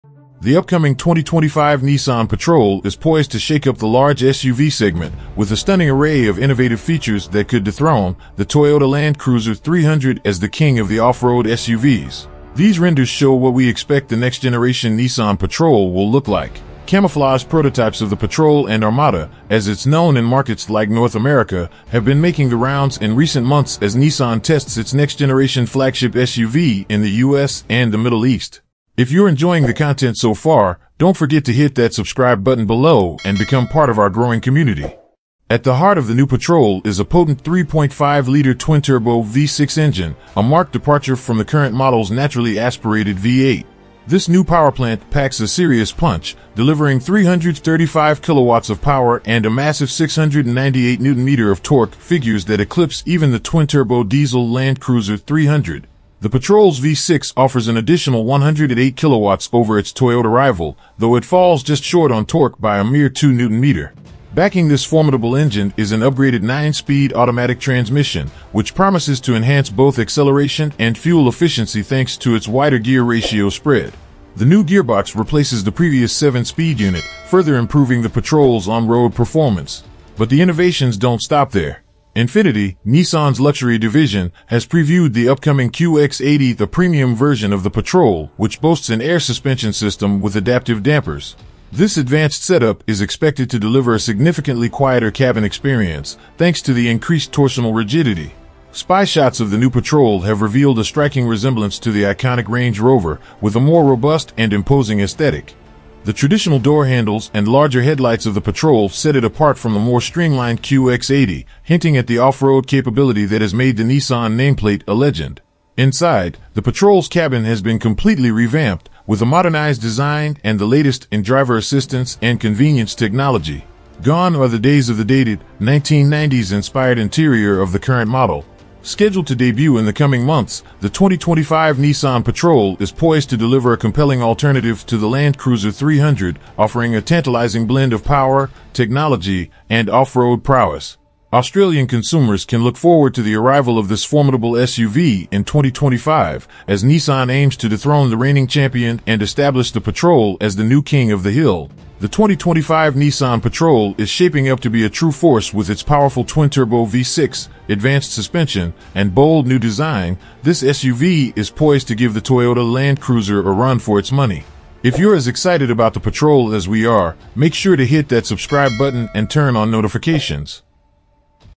2025 Nissan Patrol Models & SHOCKS Everybody- Land Cruiser Competitor!
2025 Nissan Patrol Models & SHOCKS Everybody- Land Cruiser Competitor!

Get a sneak peek at the 2025 Nissan Patrol with its innovative features that are set to rival the Land Cruiser. This SUV is a game-changer in the world of automotive technology!